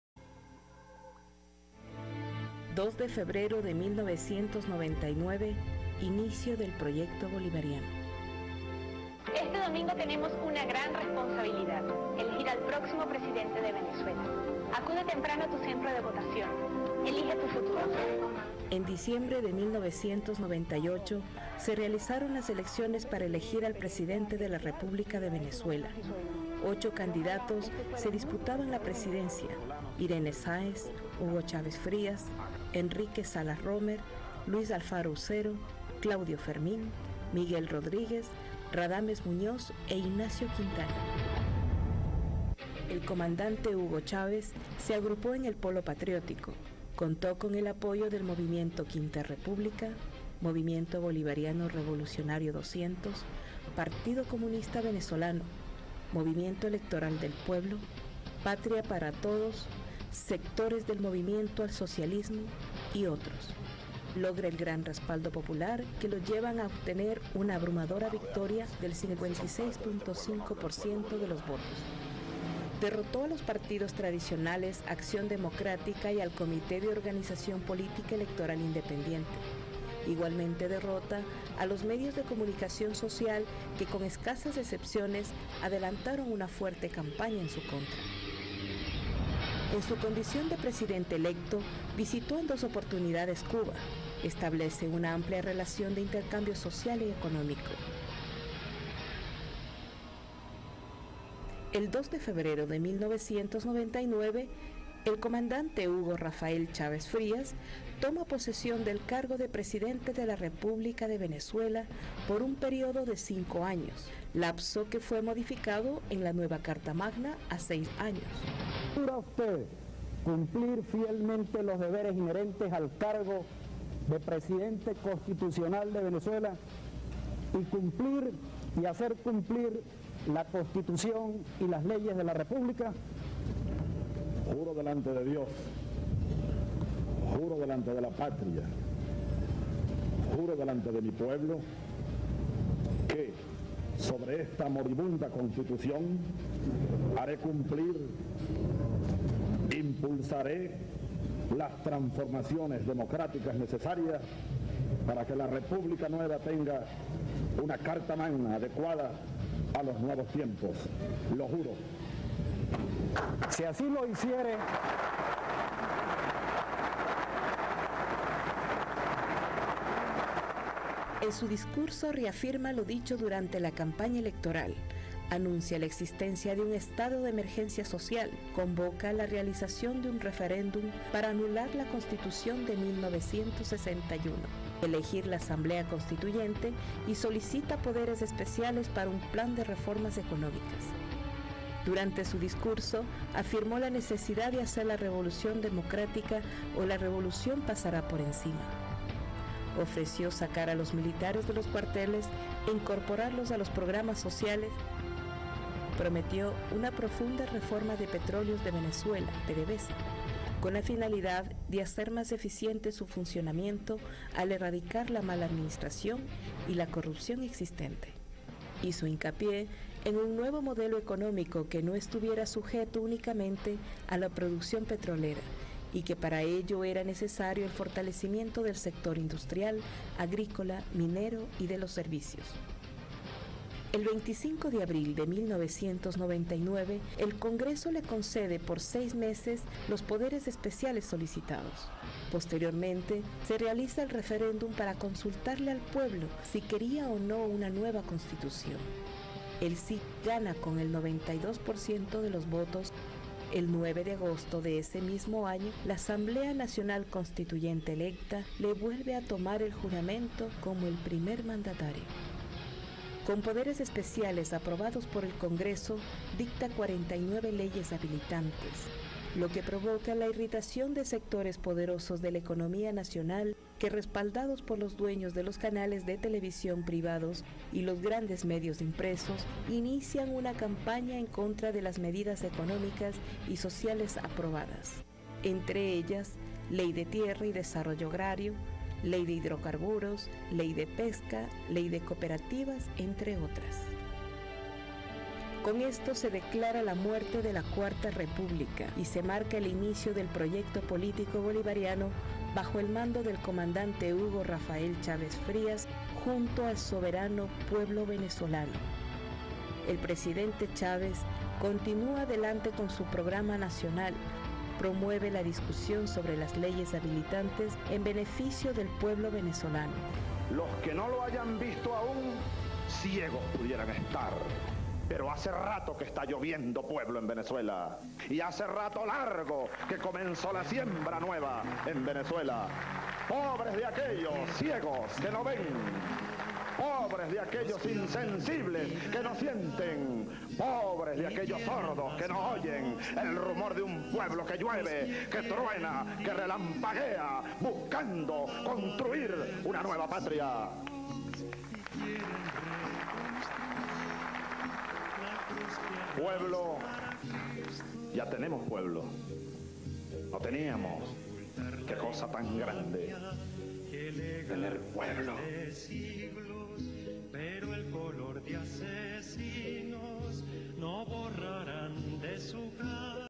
02 de Febrero de 1999 inicio del Proyecto Bolivariano
"Juro delante de Dios, juro delante de la Patria, juro delante de mi pueblo que sobre esta moribunda Constituciòn impulsaré las transformaciones democráticas necesarias para que la Repùblica nueva tenga una Carta magna adecuada a los nuievos tiempos. Lo juro". Fueron las palabras de Chávez a la hora de tomar el cargo como presidente